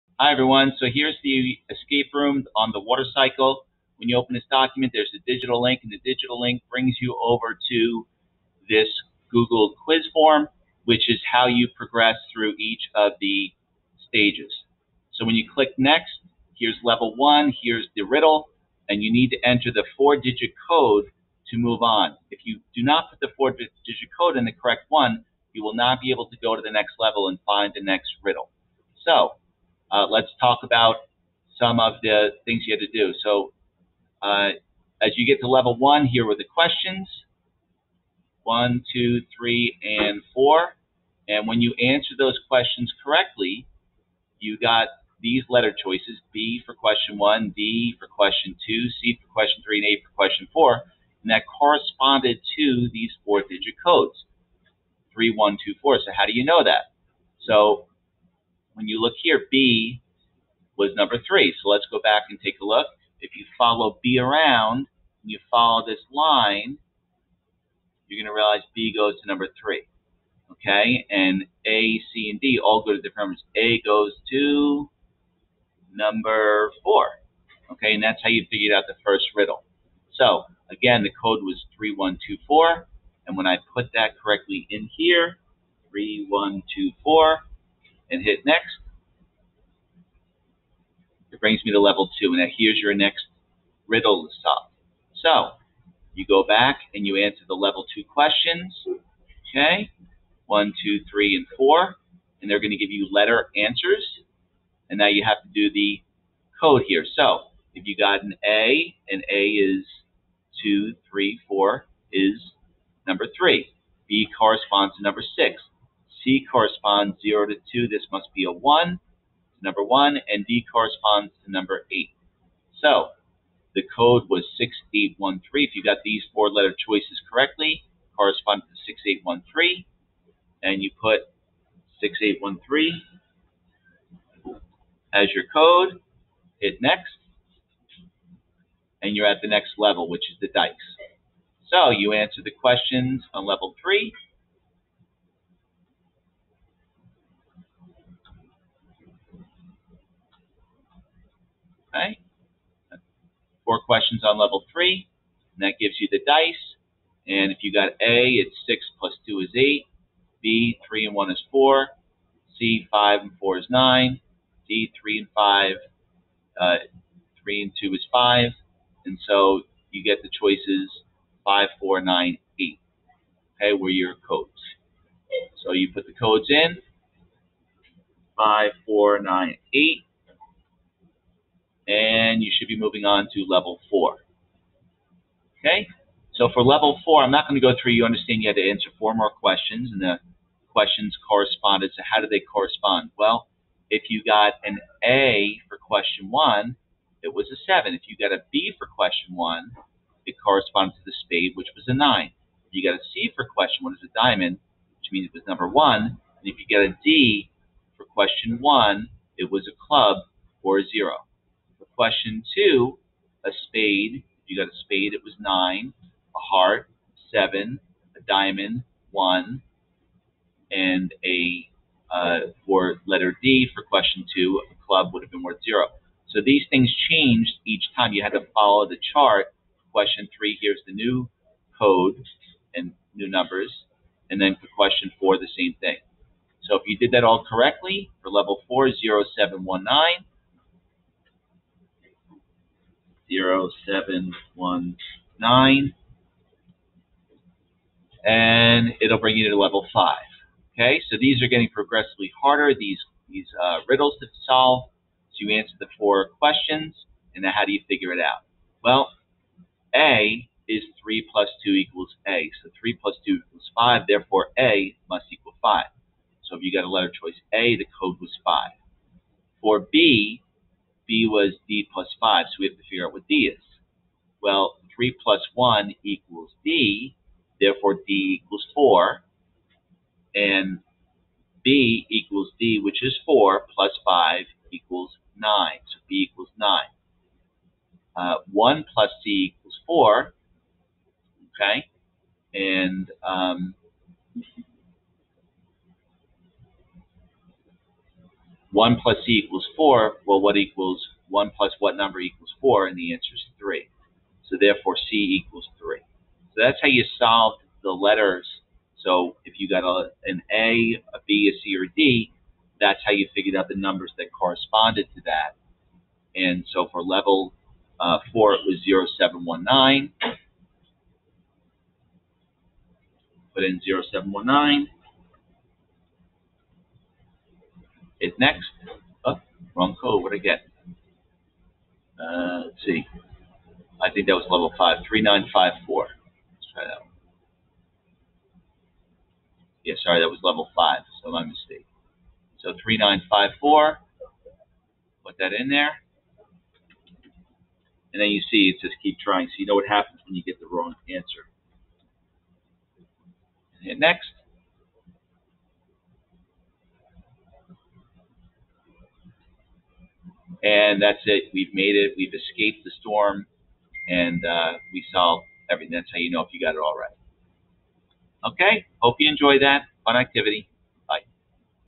Water Cycle Escape room solution: Jun 9, 2021 8:45 AM
This is a video that explains the solution set to the water cycle escape room.  Only view if you have given your best effort.  Look too soon and it will spoil the fun.